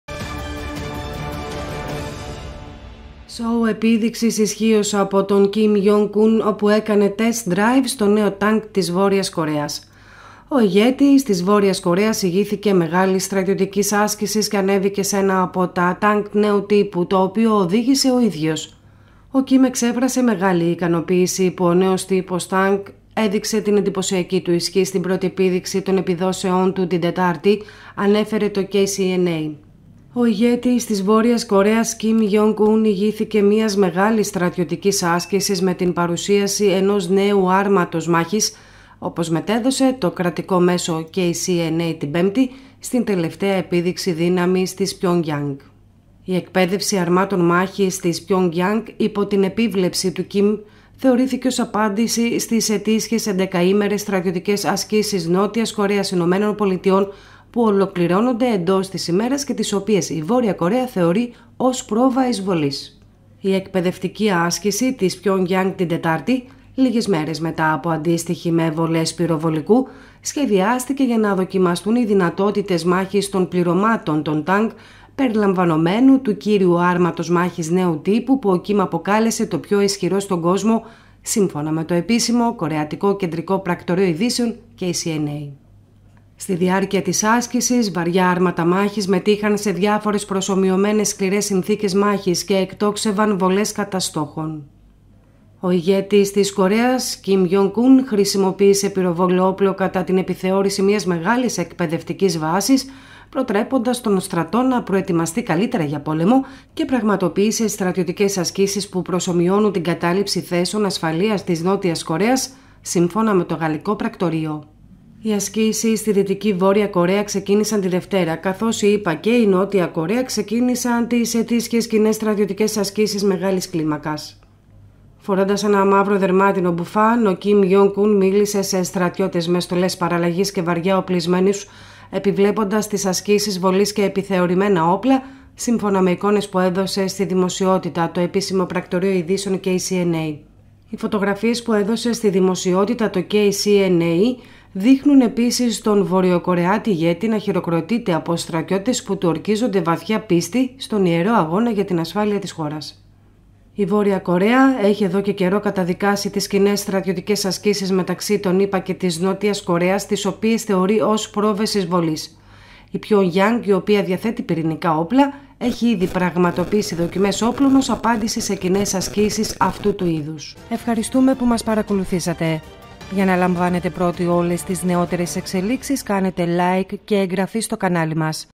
Σόου από Κιμ: Την μία μέρα με πολυβόλο την άλλη πάνω σε τανκ είναι έτοιμος να μπει στη Νότια Κορέα!
Σόου επίδειξης ισχύος από τον Κιμ Γιονγκ Ουν όπου έκανε «test drive» στο νέο τανκ της Βόρειας Κορέας. Ο ηγέτης της Βόρειας Κορέας ηγήθηκε μεγάλης στρατιωτικής άσκησης και ανέβηκε σε ένα από τα τανκ νέου τύπου το οποίο οδήγησε ο ίδιος

Ο ηγέτης της Βόρειας Κορέας Κιμ Γιονγκ Ουν χρησιμοποίησε πυροβόλο όπλο κατά την επιθεώρηση μιας μεγάλης εκπαιδευτικής βάσης, προτρέποντας τον στρατό να «προετοιμαστεί καλύτερα για πόλεμο» και πραγματοποίησε στρατιωτικές ασκήσεις που προσομοιώνουν την κατάληψη θέσεων ασφαλείας της Νότιας Κορέας, σύμφωνα με το Γαλλικό Πρακτορείο.

Η εκπαίδευση αρμάτων μάχης της Πιονγιάνγκ υπό την επίβλεψη του Κιμ θεωρήθηκε ως απάντηση στις ετήσιες 11ήμερες στρατιωτικές ασκήσεις Νότιας Κορέας-ΗΠΑ, που ολοκληρώνονται εντός της ημέρας και τις οποίες η Βόρεια Κορέα θεωρεί ως πρόβα εισβολής.

Κεφάλαια:
0:00 - Εισαγωγή
0:03 - "Test drive" Κιμ στο νέο τανκ της Βόρειας Κορέας
1:34 - Η Βόρεια Κορέα ετοιμάζεται

Ο Κιμ εξέφρασε «μεγάλη ικανοποίηση» που ο νέος τύπος τανκ έδειξε την εντυπωσιακή του ισχύ στην πρώτη επίδειξη των επιδόσεών του την Τετάρτη, ανέφερε το KCNA.

Ο ηγέτης της Βόρειας Κορέας Κιμ Γιονγκ Ουν ηγήθηκε μιας μεγάλης στρατιωτικής άσκησης με την παρουσίαση ενός νέου άρματος μάχης, όπως μετέδωσε το κρατικό μέσο KCNA την Πέμπτη, στην τελευταία επίδειξη δύναμης της Πιονγιάνγκ.

Η εκπαιδευτική άσκηση της Πιονγιάνγκ την Τετάρτη - λίγες μέρες μετά από αντίστοιχη με βολές πυροβολικού - σχεδιάστηκε για να δοκιμαστούν οι δυνατότητες μάχης των πληρωμάτων των τανκ, περιλαμβανομένου του κύριου άρματος μάχης νέου τύπου που ο Κιμ αποκάλεσε «το πιο ισχυρό στον κόσμο», σύμφωνα με το επίσημο Κορεατικό Κεντρικό Πρακτορείο Ειδήσεων (KCNΑ). Στη διάρκεια της άσκησης βαριά άρματα μάχης μετείχαν σε διάφορες προσομοιωμένες σκληρές συνθήκες μάχης και εκτόξευαν βολές κατά στόχων.

Η Βόρεια Κορέα ετοιμάζεται

Οι ασκήσεις στη δυτική Βόρεια Κορέα ξεκίνησαν τη Δευτέρα καθώς οι Ηνωμένες Πολιτείες και η Νότια Κορέα ξεκίνησαν τις ετήσιες κοινές στρατιωτικές ασκήσεις μεγάλης κλίμακας.

Φορώντας ένα μαύρο δερμάτινο μπουφάν, ο Κιμ Γιονγκ Ουν μίλησε σε στρατιώτες με στολές παραλλαγής και «βαριά οπλισμένους», επιβλέποντας τις ασκήσεις βολής και επιθεωρημένα όπλα, σύμφωνα με εικόνες που έδωσε στη δημοσιότητα το επίσημο πρακτορείο ειδήσεων KCNA.

Οι φωτογραφίες που έδωσε στη δημοσιότητα το KCNA δείχνουν επίσης τον Βορειοκορεάτη ηγέτη να χειροκροτείται από στρατιώτες που του «ορκίζονται βαθιά πίστη» στον «ιερό αγώνα για την ασφάλεια της χώρας».

Η Βόρεια Κορέα έχει εδώ και καιρό καταδικάσει τις κοινές στρατιωτικές ασκήσεις μεταξύ των Ηνωμένων Πολιτειών και της Νότιας Κορέας, τις οποίες θεωρεί ως πρόβες εισβολής. Η Πιονγκγιάνγκ, η οποία διαθέτει πυρηνικά όπλα, έχει ήδη πραγματοποιήσει δοκιμές όπλων ως απάντηση σε κοινές ασκήσεις αυτού του είδους.